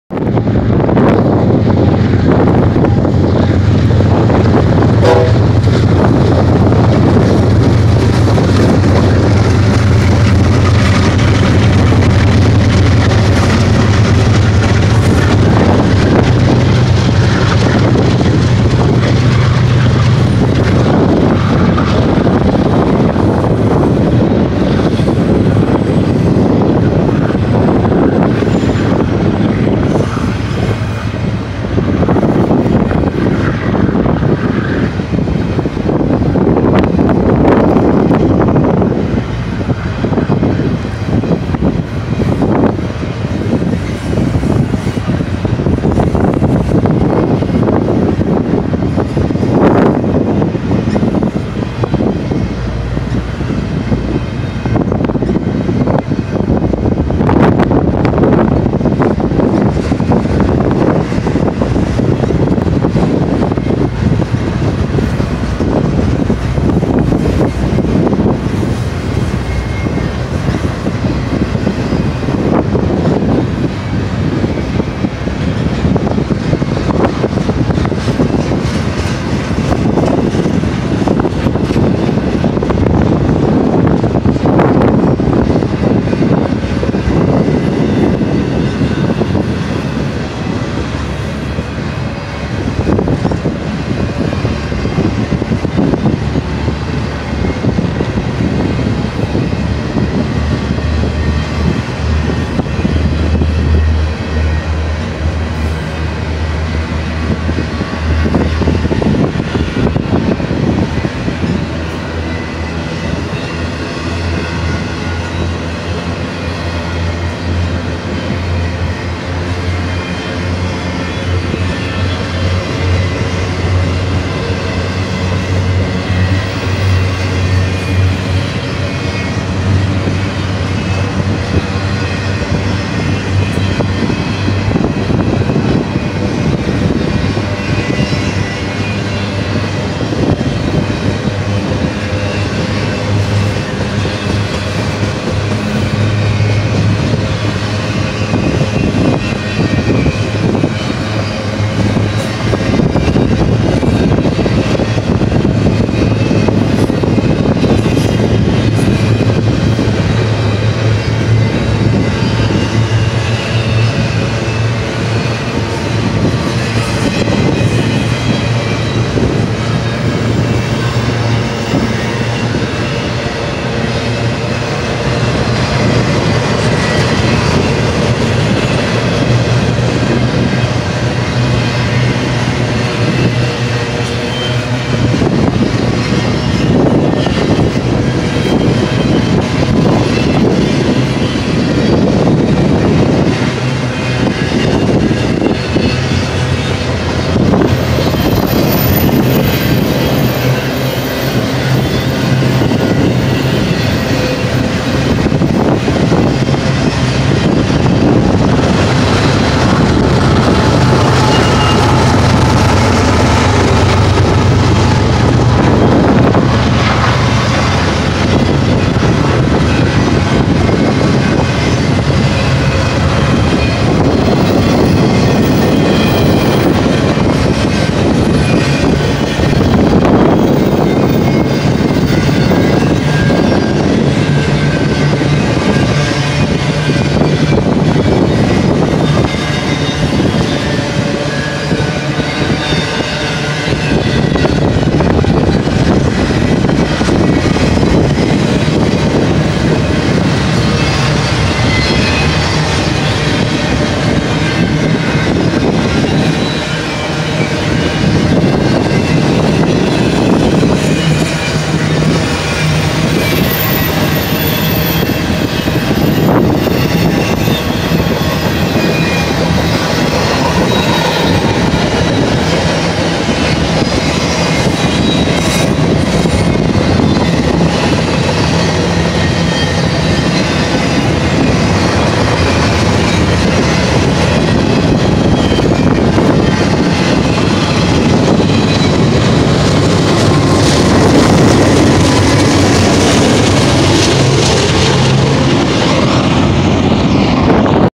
Monster WB BNSF Domestic Intermodal/Port Stack Train At Lenwood Ca!
I caught this monster westbound BNSF domestic intermodal/port stack train feat duo mid DPUs and duo rear DPUs passing by here in Lenwood Ca!

6783-ES44C4 (BNSF) (Leading Unit)
6522-ES44C4 (BNSF) (Trailing)
Middle Engines
3904-ET44C4 (Tier 4) (BNSF) (Mid DPU)
4258-ES44C4 T4 Credit locos (BNSF) (Mid DPU)
Rear Engines
5078-Dash-9C44-9W (BNSF) (Rear DPU)
4171-Dash-9C44-9W (BNSF) (Rear DPU)